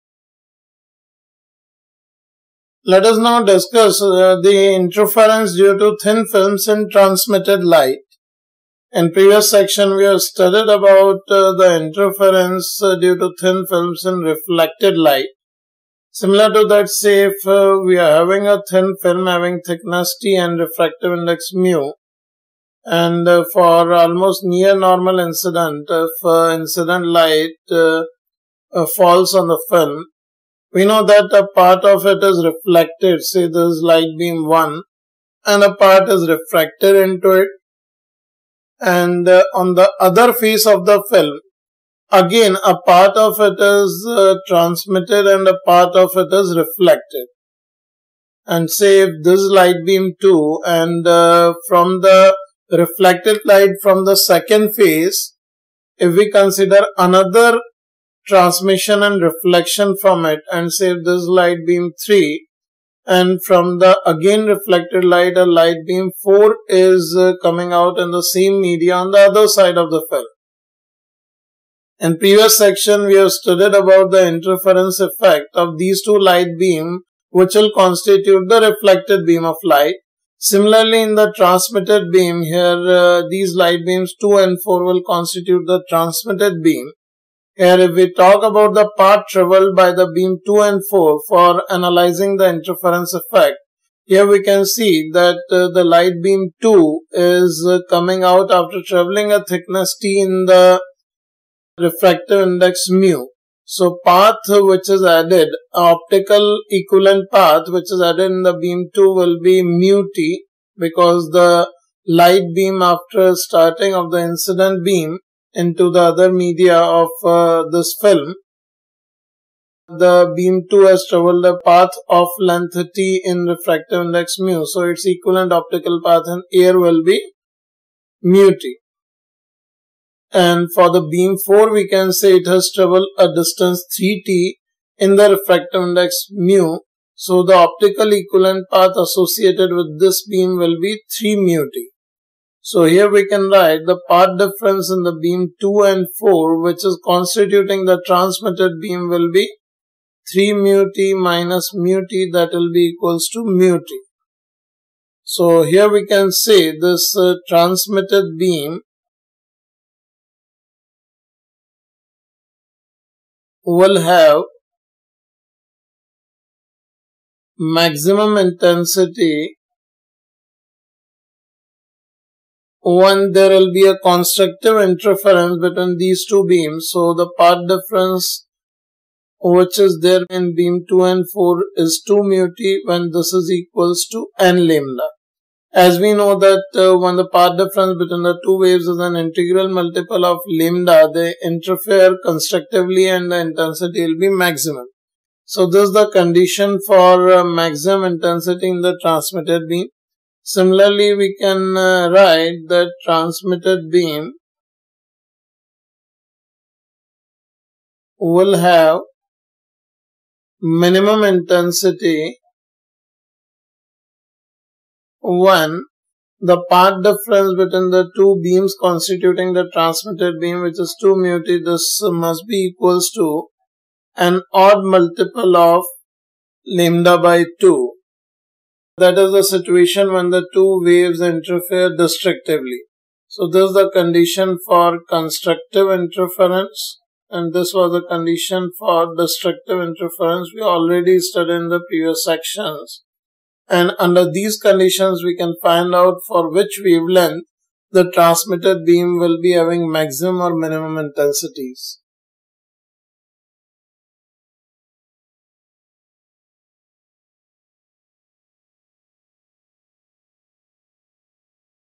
Class 12 Physics | Interference |#40 Interference due to Thin Films in Transmitted Light |JEE & NEET
PG Concept Video | Interference of Light | Interference due to Thin Films in Transmitted Light by Ashish Arora

Students can watch all concept videos of class 12 Interference of Light for jee & neet in proper sequence through PG channel playlist.